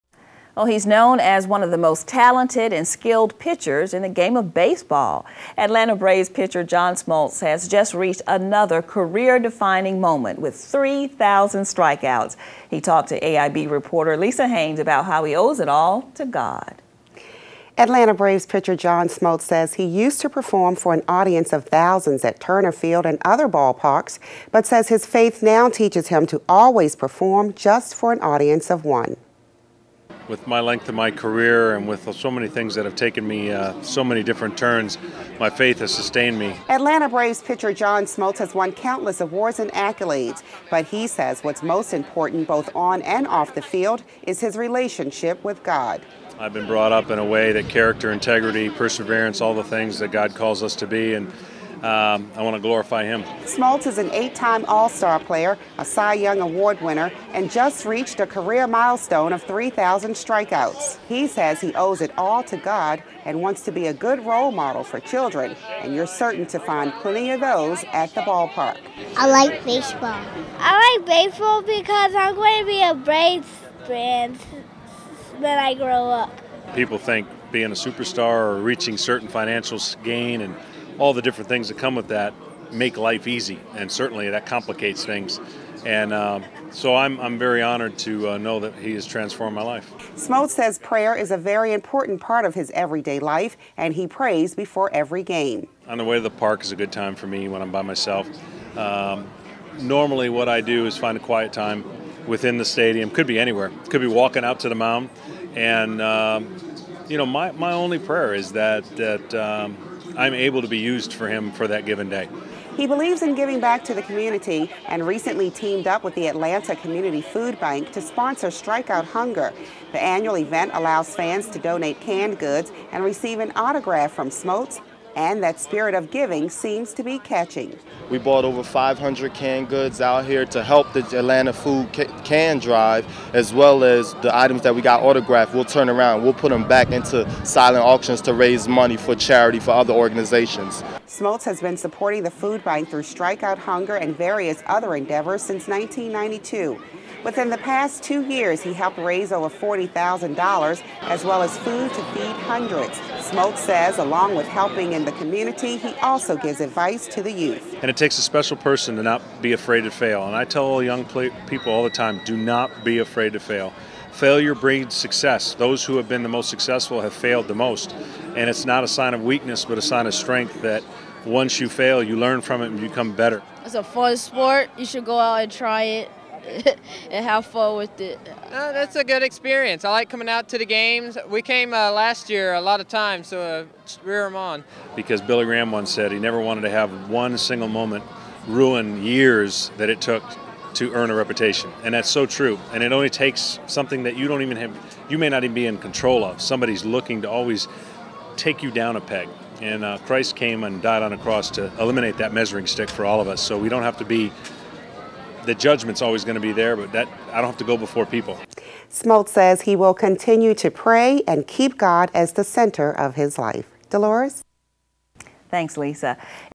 John Smoltz Story.mov
Story I covered with reporter Lisa Haynes. John Smoltz has a non profit charity that donates food to the homeless. John Smoltz made a appearance for his non-profit organization before a Atlanta Braves game. He offered his autograph for any fan who donated 2 canned goods towards his charity. The story appeared on AIB NEWS and INFOCUS (programs that air on Atlanta Interfaith Broadcasters)